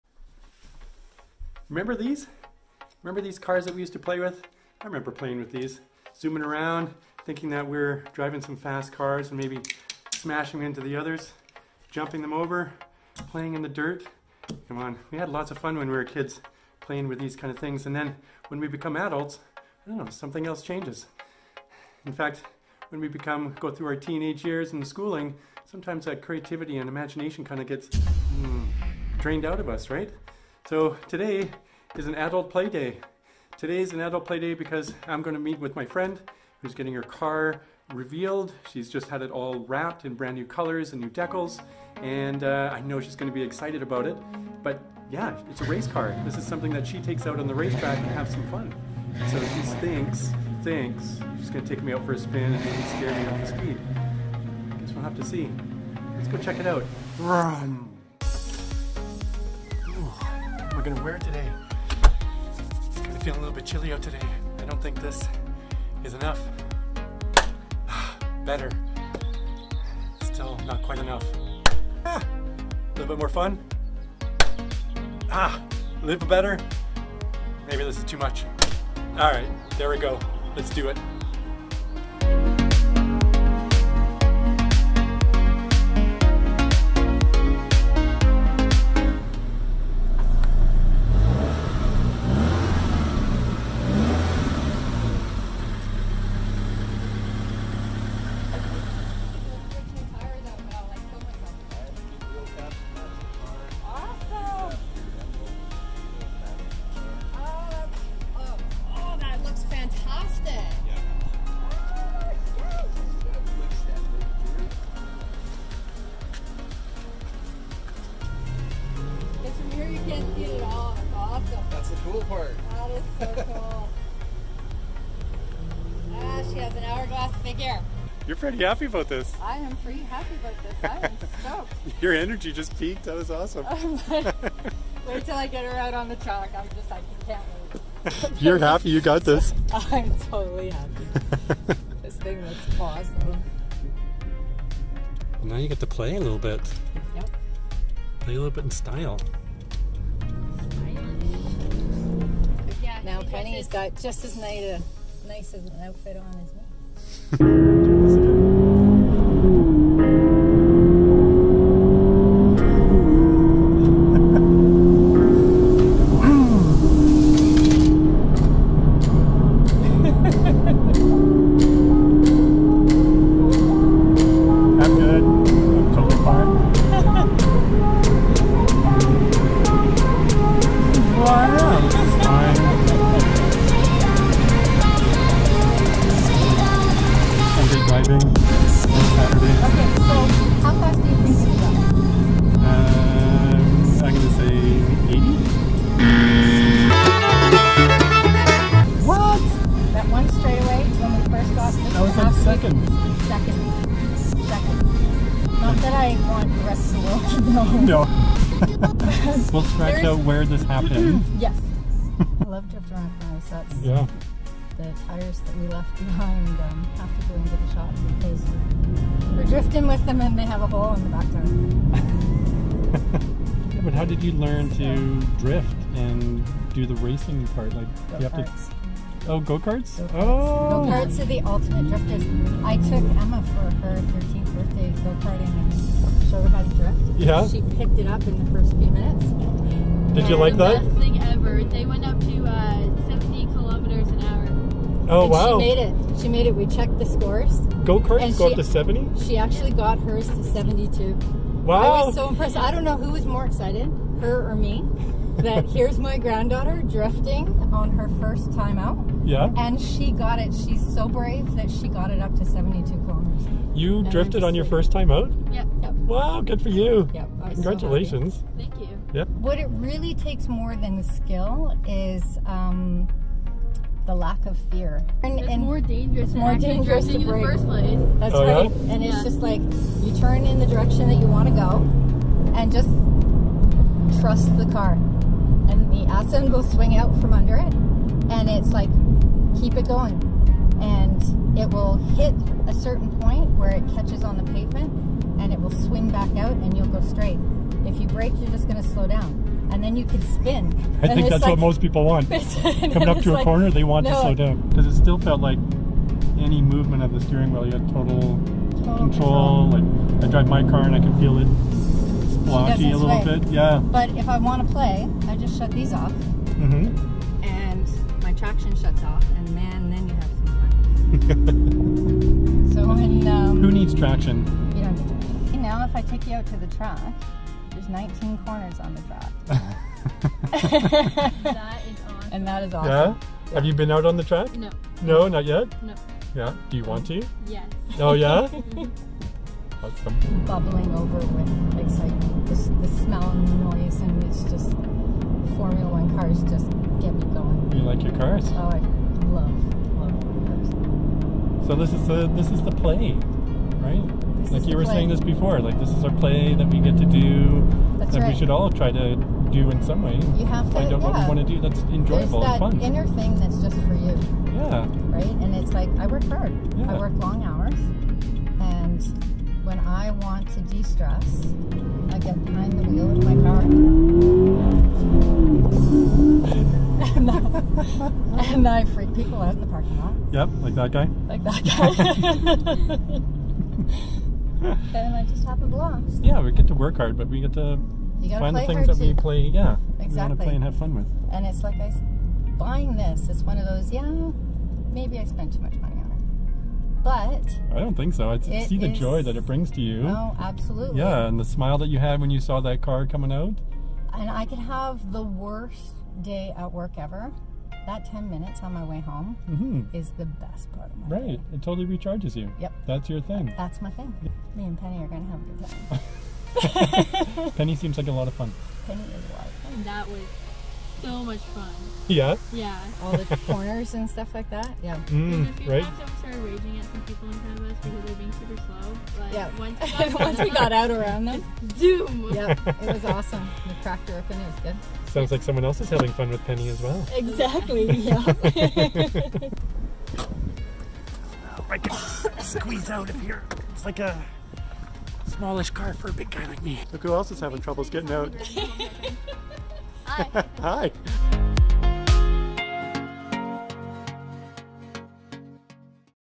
Speeding In A "Real" Race Car
Went on a fast drive in a race car and found what many are missing.

(the school I started)